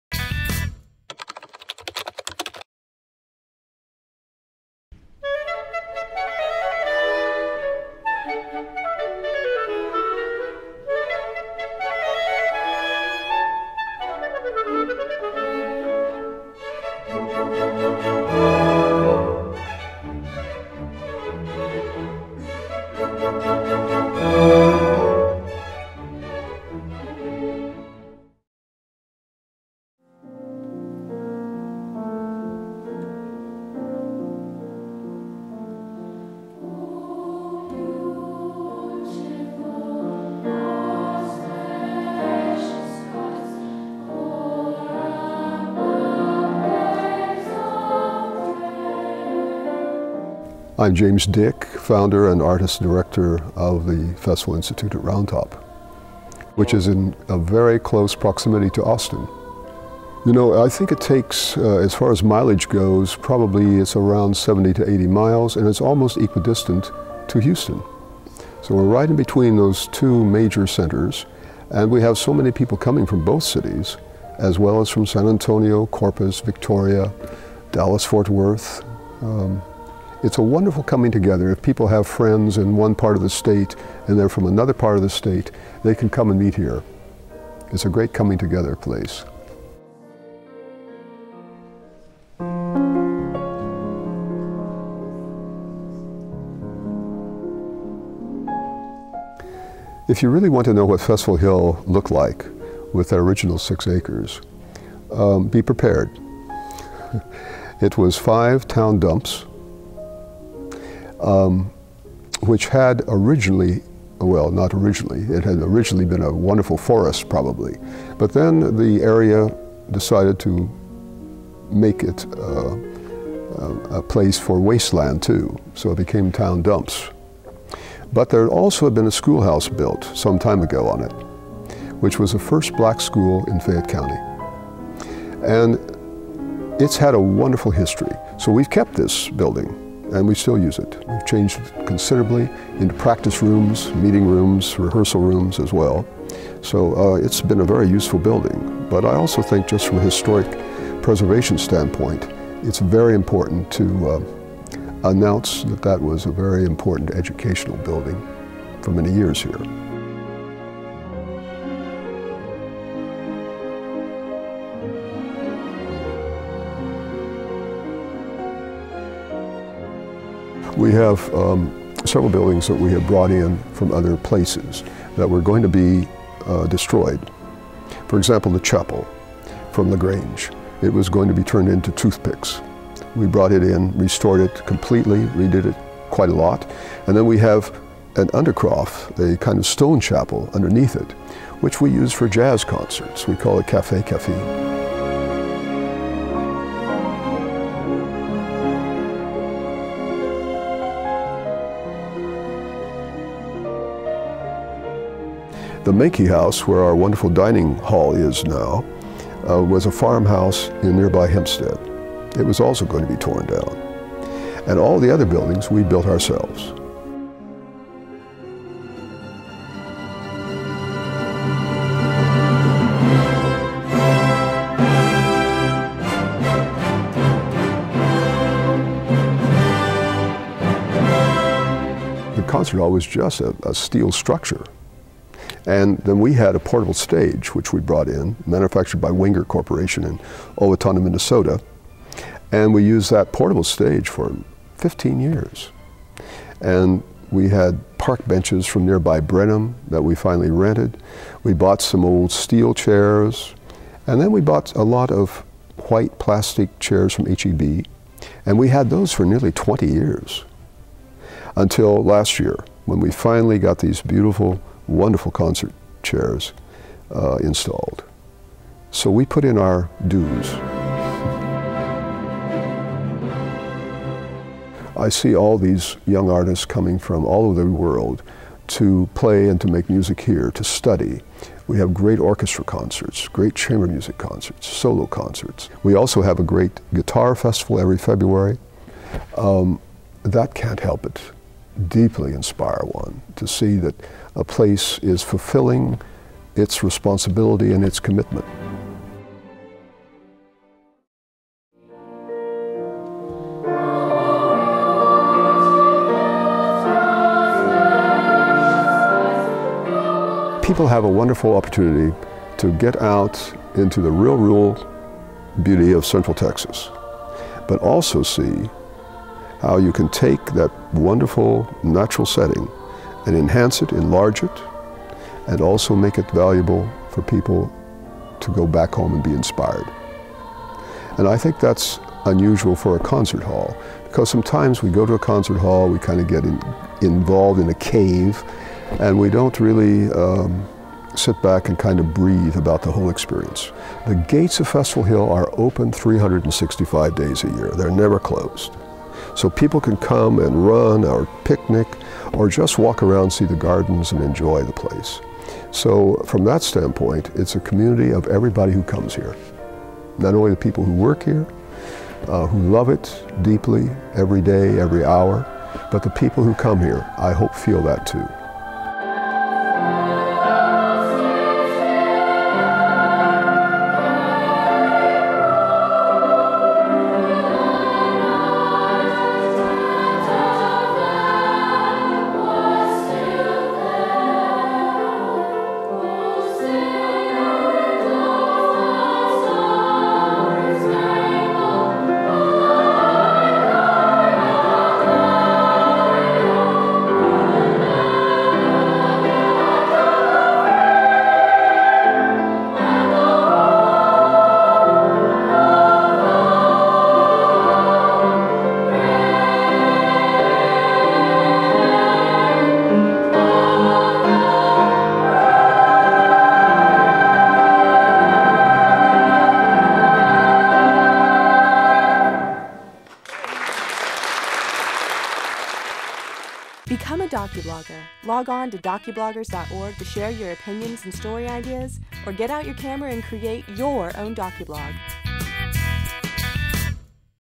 Festival Hill
Nestled among lush green forests in Central Texas, the International Festival-Institute at Round Top has been operating for nearly 40 years. Their 200 plus acre campus has been a training ground for some of the world's most gifted musicians, and has been host to beautiful performances framed by the glory of Texas' natural wonders. Beyond music, Festival Hill is just a great place to explore. They have award-winning gardens, historic buildings, and old-world craftsmanship on display. The best part is that Round Top is just a short day trip from most major cities in Texas. Festival Hill is truly a must-see hidden treasure of our state.